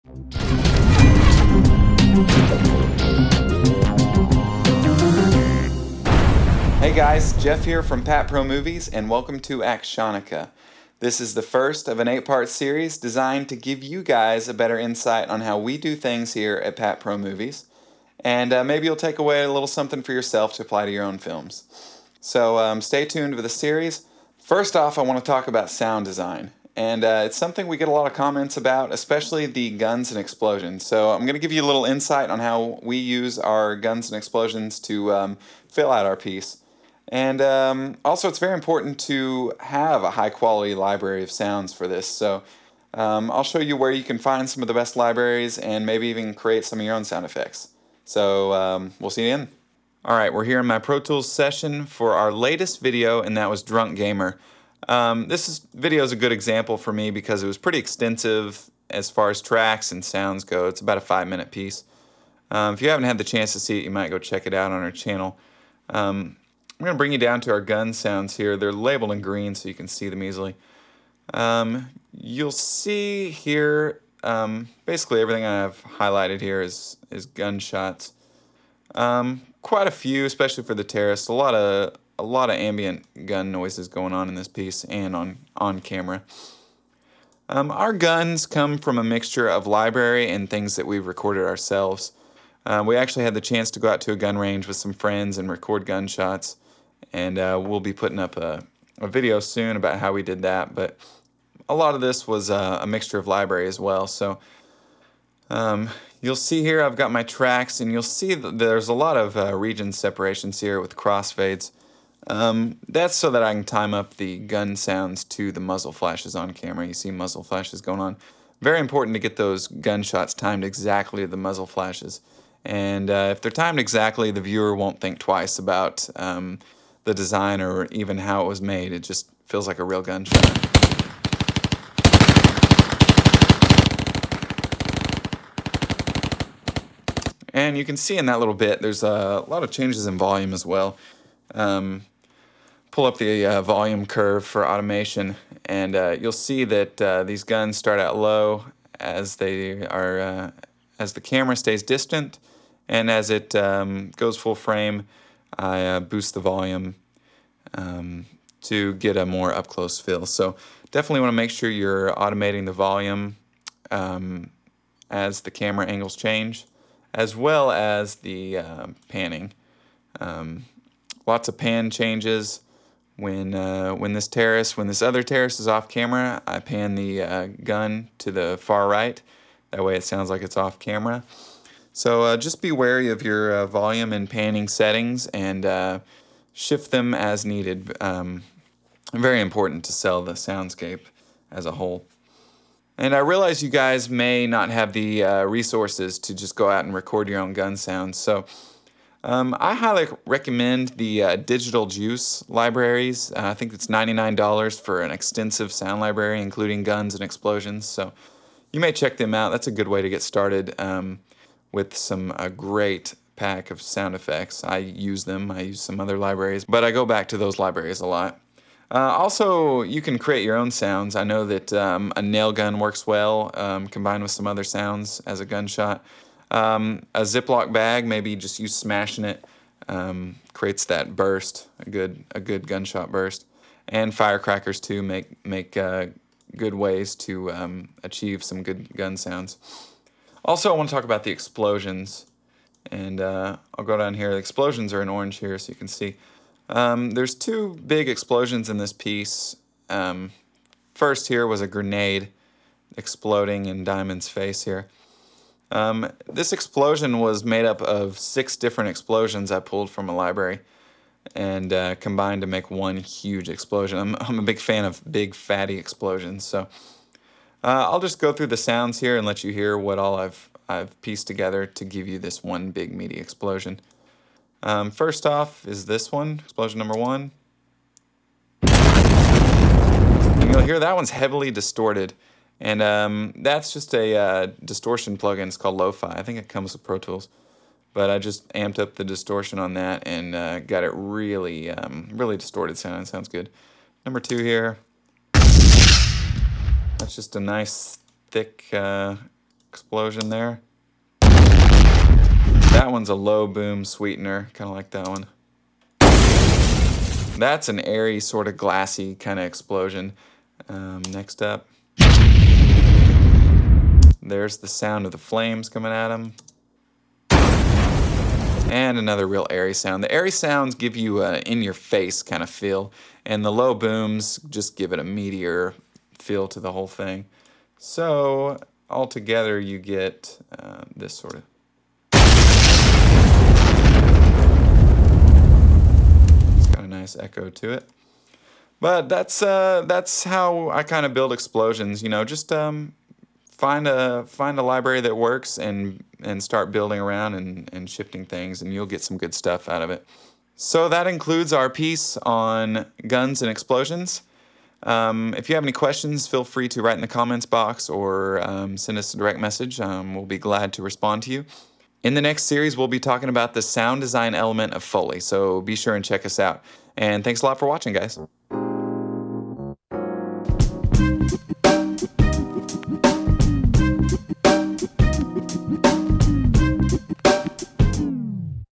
Actionakkuh Day 1 - "Sound Design: Guns and Explosions"
Day 1 of 8 of Tips and Tutorials on how we create sound design, music, and visual FX for our productions.  In this episode Jeff discusses great tips on sound designing for Guns and Explosions inside of Pro Tools.  However, these techniques can be applied to any DAW.  Questions and comments are always welcome!